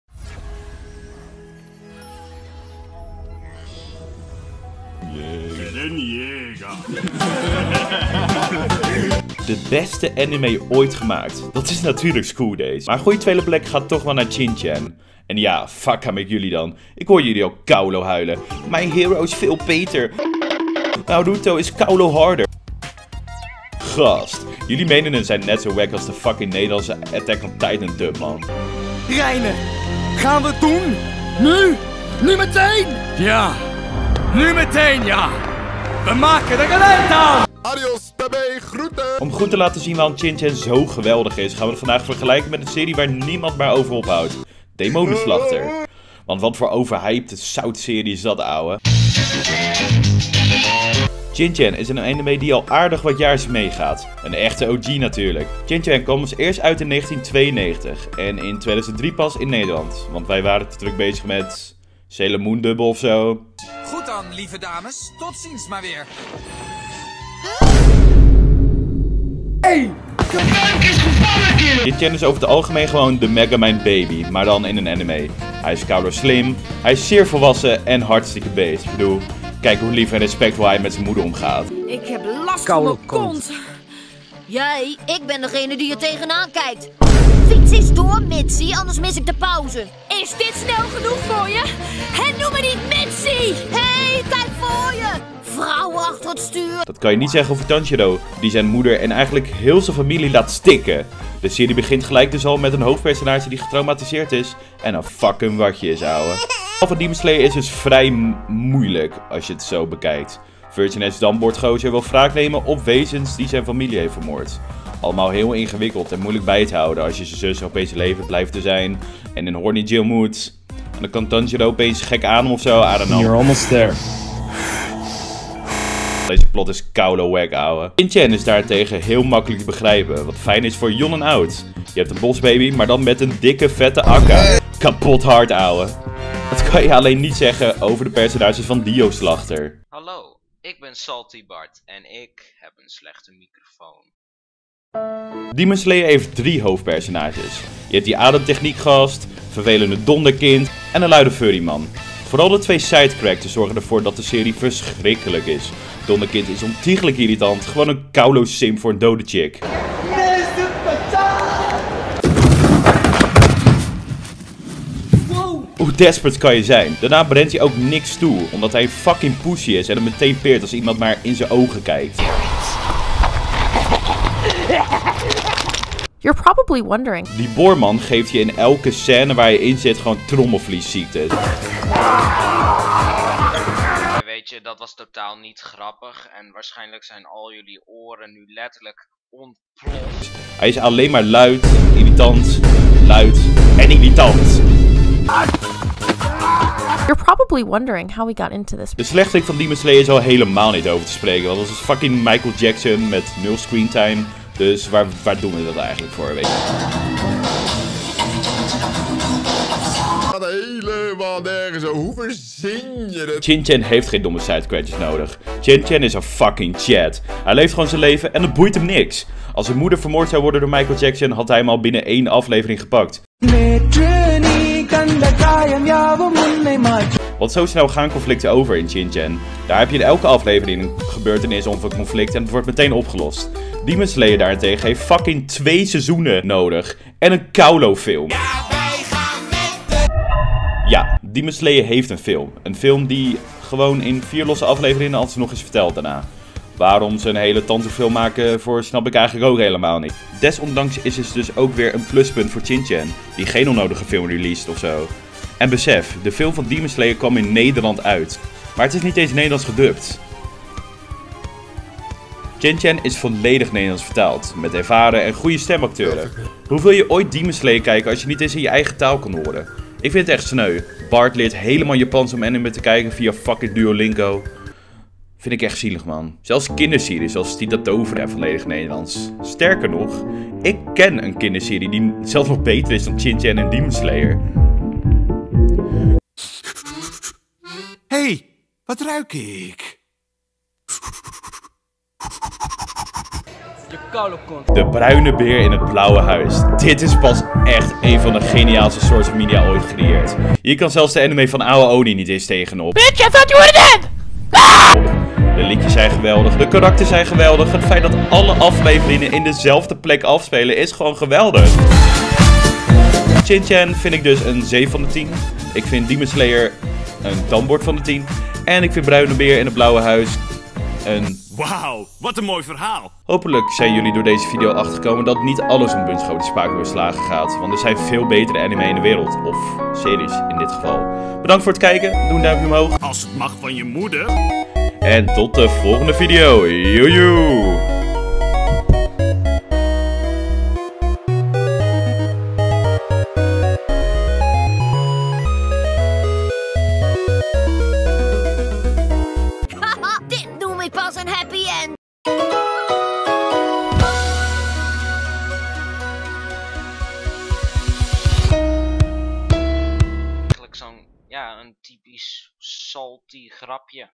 Shin Chan is een BETERE ANIME dan Demon Slayer!!!!
Bedankt voor het kijken naar mijn serieuze, hardgewerkte spreekbeurt video essay gebeuren. Neem dit serieus want het is serieus!

Demon Slayer F tier?